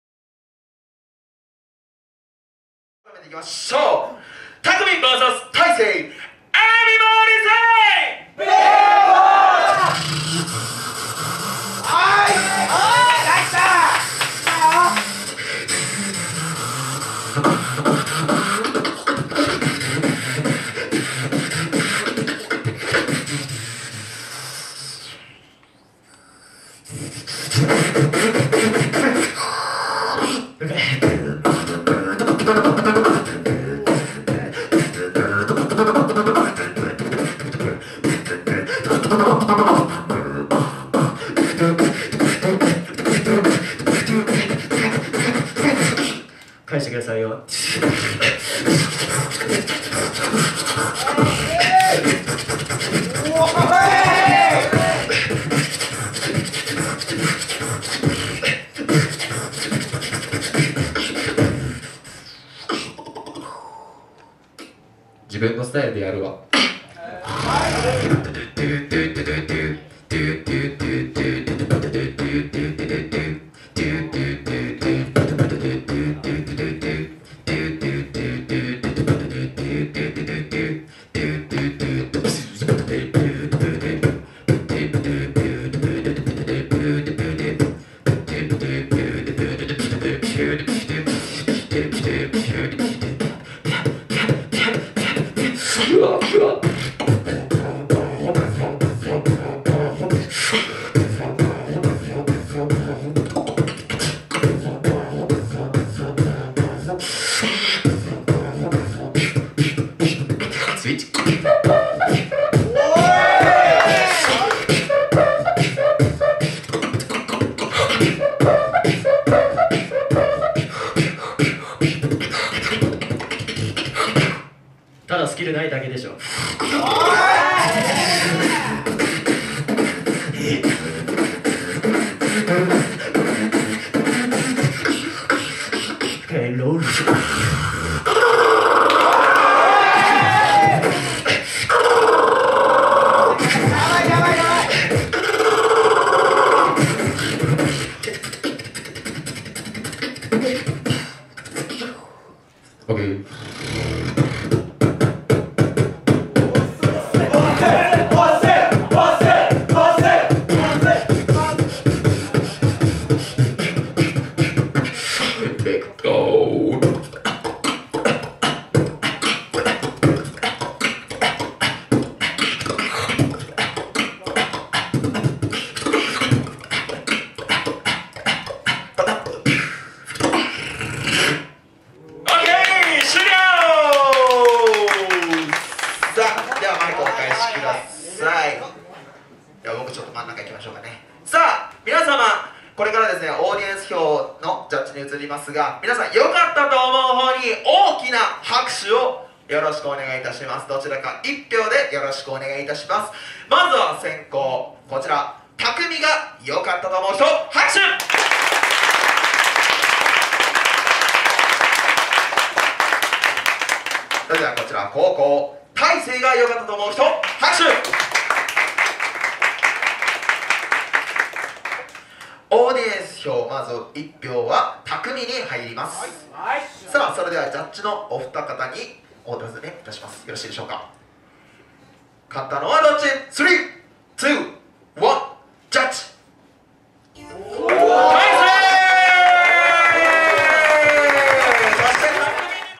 Boost福岡大会vol.2 BEST8 T@Kumi vs TAISEI
2017.7/22
MONOGRAM

JUDGE：SiM / large-K

プレイヤーだけではなくスタッフ、オーガナイザー、ジャッジなど全ての若手ビートボクサーを応援するプロジェクトイベント「Boost」